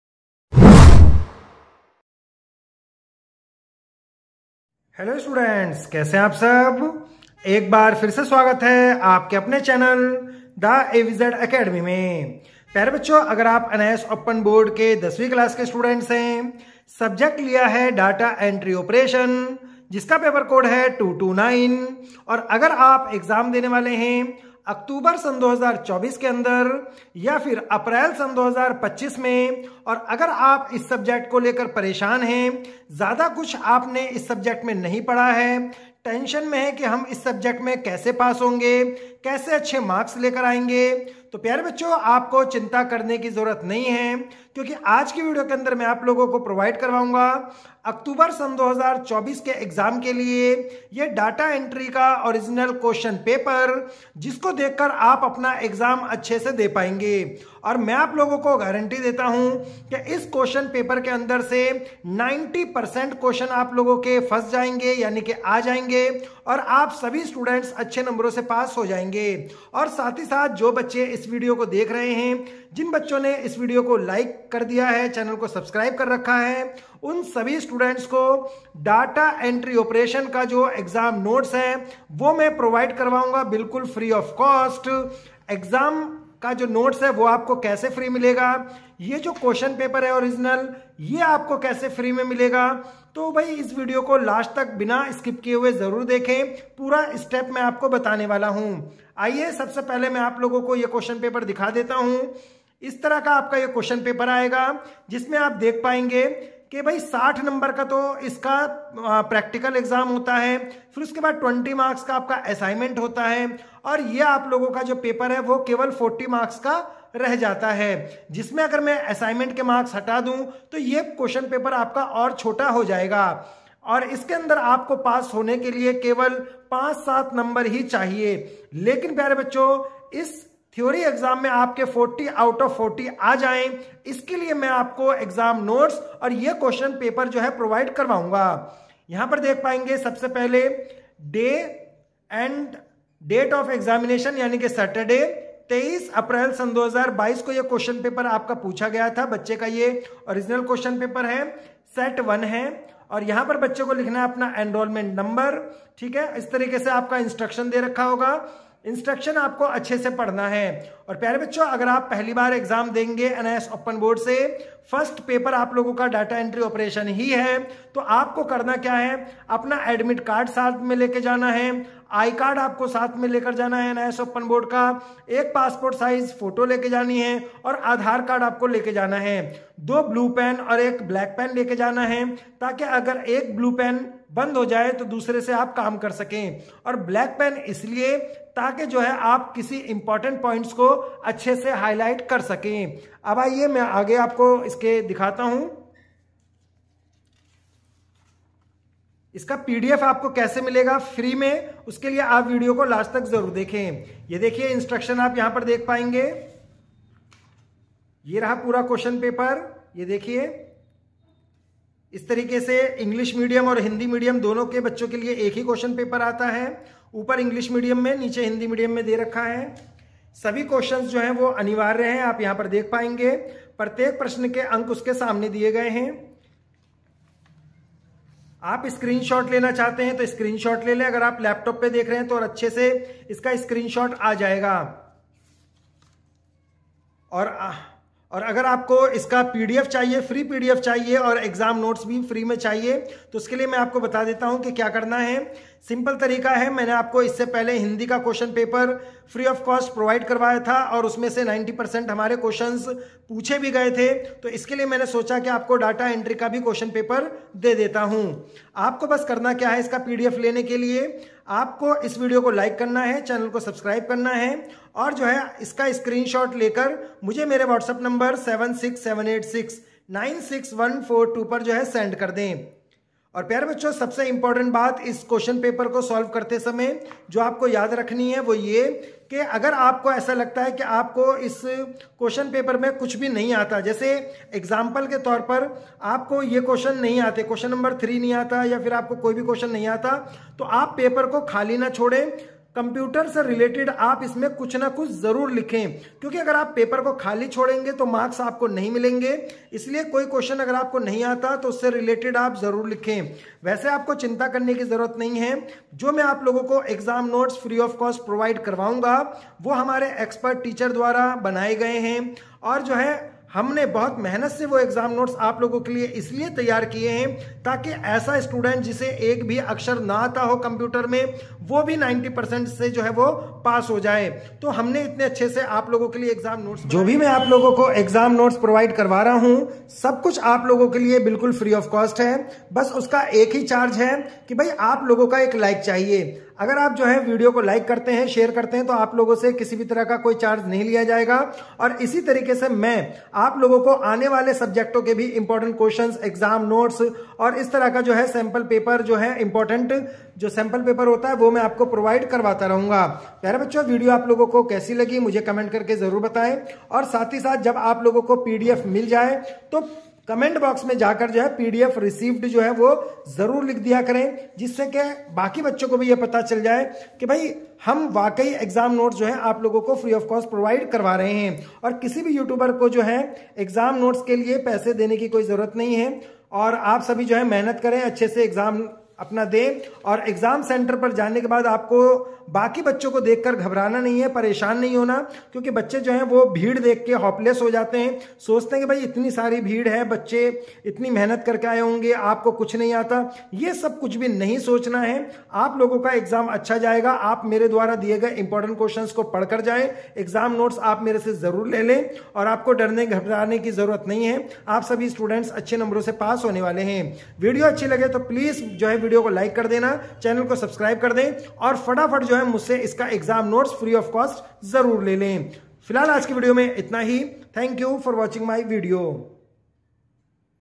NIOS Data Entry Operation (229) New Questions Paper October 2024 || यही पेपर आएगा एग्जाम में
Free  एग्जाम नोट्स का पीडीएफ लेने के लिए अभी व्हाट्सएप करें

फ्री एग्जाम नोट्स का पीडीएफ लेने के लिए अभी व्हाट्सएप करें
नमस्ते दोस्तों! 🙏

इस वीडियो में हम NIOS Data Entry Operation (229) के नए प्रश्न पत्र (October 2024) के बारे में चर्चा करेंगे। यह वही पेपर है जो आपके एग्जाम में आने वाला है। इस वीडियो में आपको महत्वपूर्ण प्रश्न और उनके उत्तर मिलेंगे जो आपकी परीक्षा की तैयारी में मदद करेंगे।

🔹 **वीडियो में शामिल विषय:**
1. महत्वपूर्ण प्रश्न और उत्तर
2. परीक्षा की तैयारी के टिप्स
3. पिछले साल के प्रश्न पत्रों का विश्लेषण
4. NIOS Data Entry Operation (229) के महत्वपूर्ण नोट्स

धन्यवाद! 😊